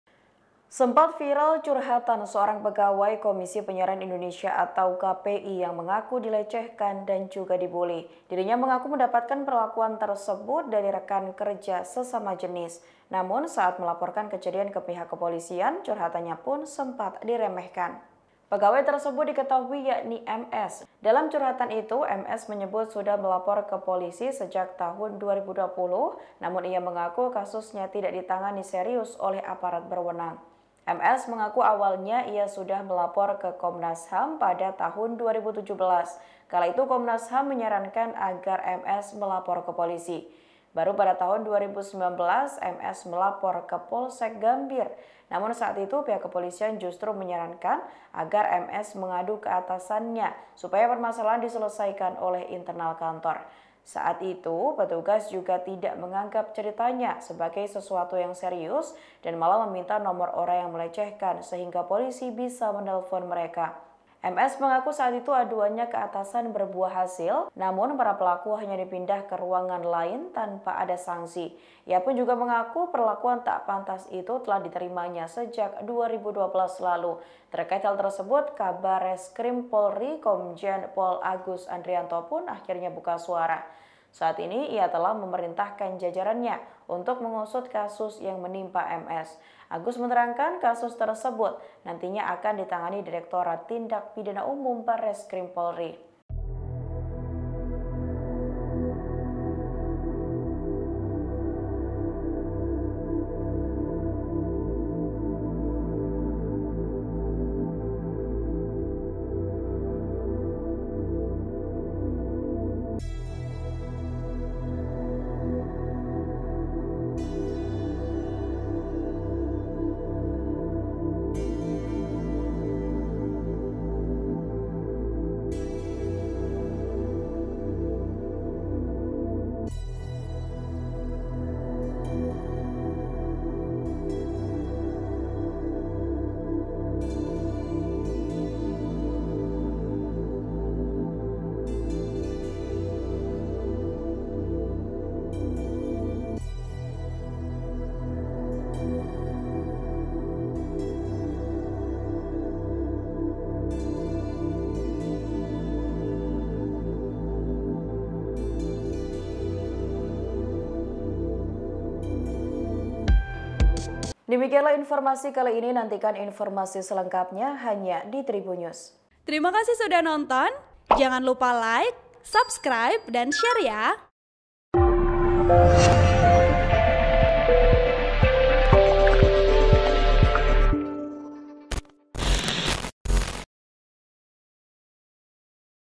Pegawai KPI yang Alami Pelecehan Sesama Jenis Mengaku Ceritanya Diremehkan Polisi, Polri Buka Suara
TRIBUN-VIDEO.COM - Sempat viral curhatan seorang pegawai Komisi Penyiaran Indonesia (KPI) yang mengaku dilecehkan dan dibully.

Dirinya mengaku mendapatkan perlakuan tersbeut dari rekan kerja sesama jenis.

Namun, saat melaporkan kejadian ke pihak kepolisian, curhatannya pun sempat diremehkan.

Pegawai tersebut diketahui yakni MS.

Dalam curhatan itu, MS menyebut sudah melapor ke polisi sejak tahun 2020 namun ia mengaku kasusnya tidak ditanggapi serius oleh aparat berwenang.

MS mengaku awalnya ia sudah melapor ke Komnas HAM pada tahun 2017.

Kala itu Komnas HAM menyarankan agar MS melapor ke polisi.

Baru pada tahun 2019, MS melapor ke Polsek Gambir.

Namun saat itu pihak kepolisian justru menyarankan agar MS mengadu ke atasannya supaya permasalahan diselesaikan oleh internal kantor.

Saat itu petugas juga tidak menganggap ceritanya sebagai sesuatu yang serius dan malah meminta nomor orang yang melecehkan sehingga polisi bisa menelepon mereka.

MS mengaku saat itu aduannya ke atasan berbuah hasil.

Namun para pelaku hanya dipindahkan ke ruangan lain tanpa ada sanksi.